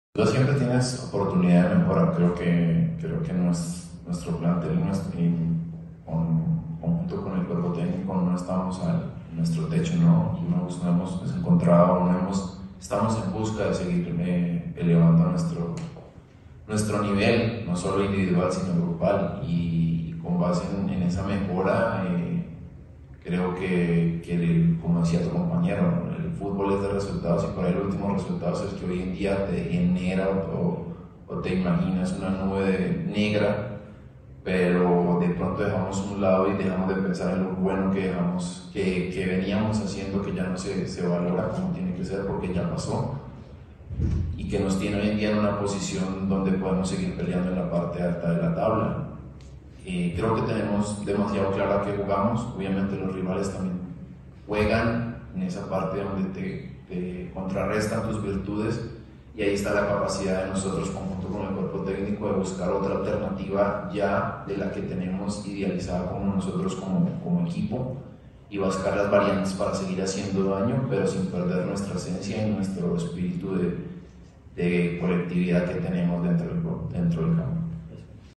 Habla Camilo Vargas sobre el momento que vive Atlas tras la derrota ante Gallos
Habla Camilo Vargas sobre el momento que vive Atlas tras la derrota ante Gallos, de cara a la visita ante Toluca. 🦊⚽